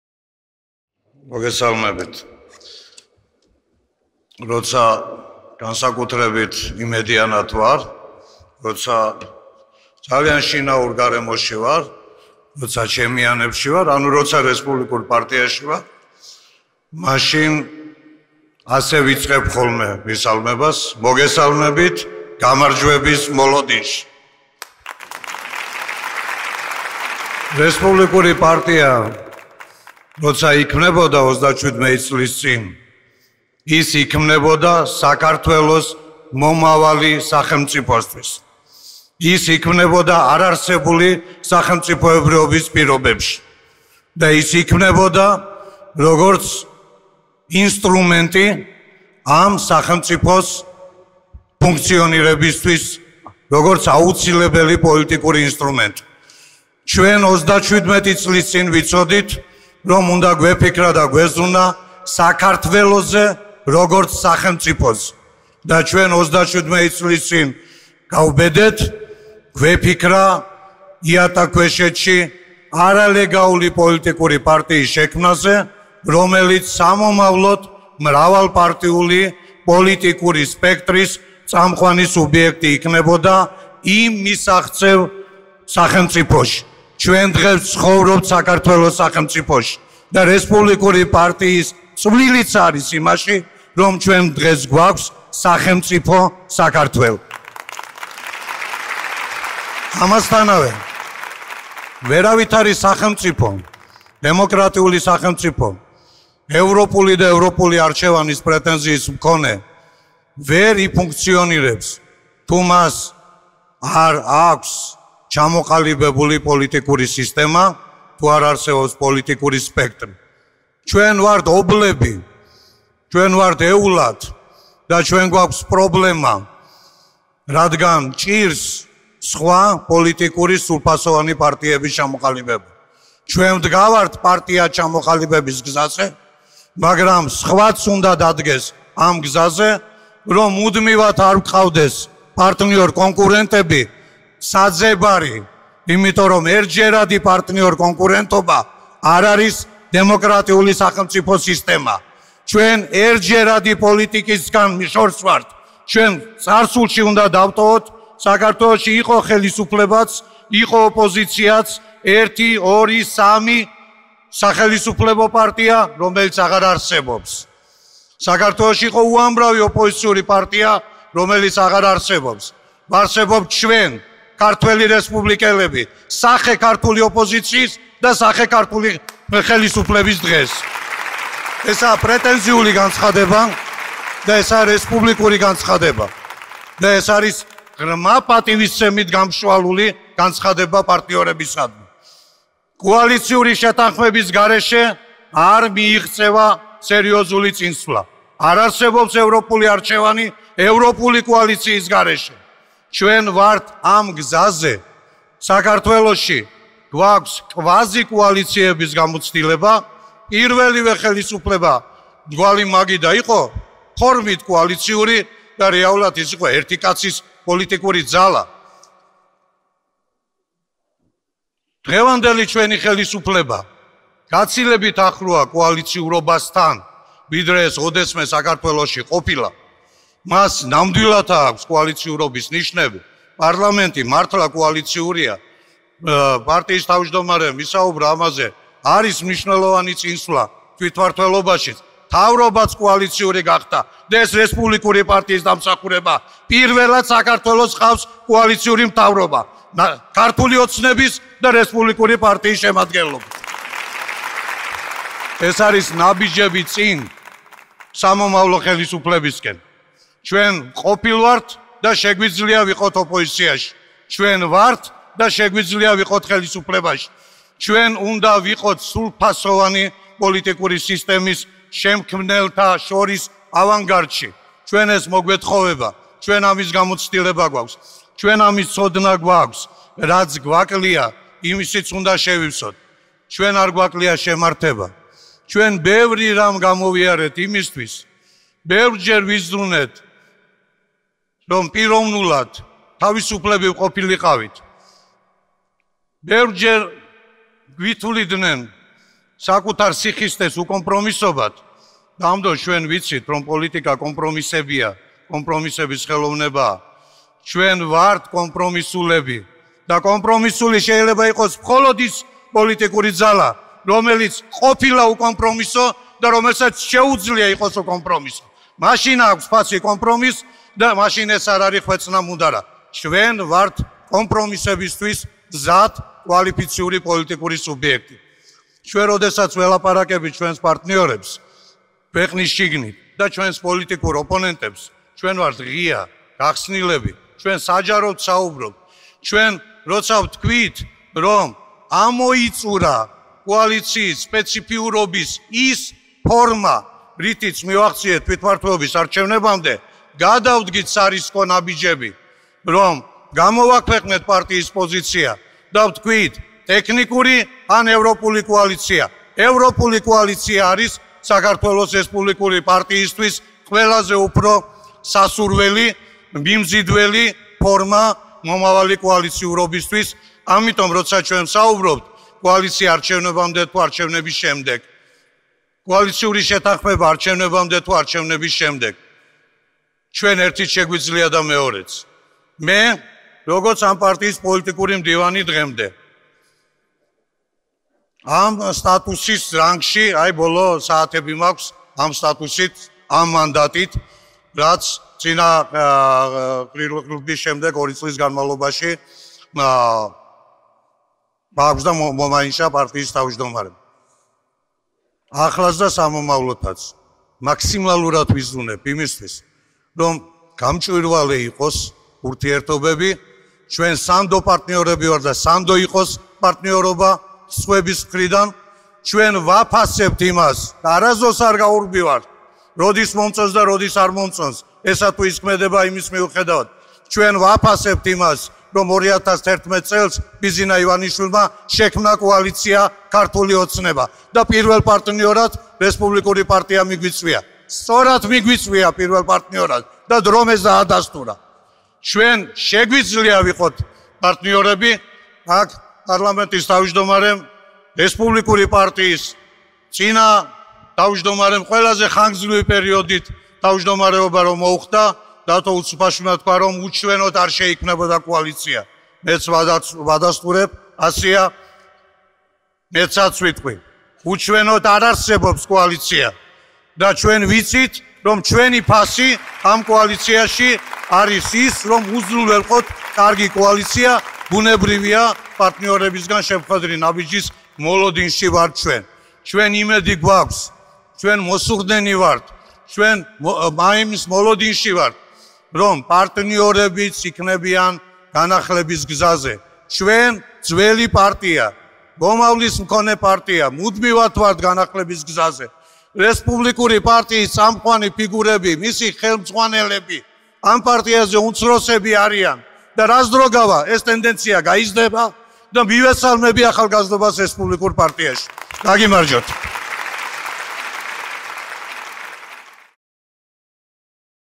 დავით ბერძენიშვილის სიტყვა რესპუბლიკელების XVI ყრილობაზე (06.12.2015)
დავით ბერძენიშვილი: "ჩვენ ვართ დიდი წარსულისა და მომავლის პარტია.

37 წლის წინ, მაშინ როდესაც იქმნებოდა რესპუბლიკური პარტია, ის იქმნებოდა საქართველოს, როგორც სახელმწიფოს ფუნქციონირებისათვის. ჩვენ ბევრი რამ გამოვიარეთ იმისათვის, რომ პიროვნულად თავისუფლები ვყოფილიყავით. დღეს, ჩვენ მრავალპარტიული სპექტრის წამყვანი სუბიექტები ვართ. ჩვენ ვართ სანდო პარტნიორები და დრომ ეს დაადასტურა".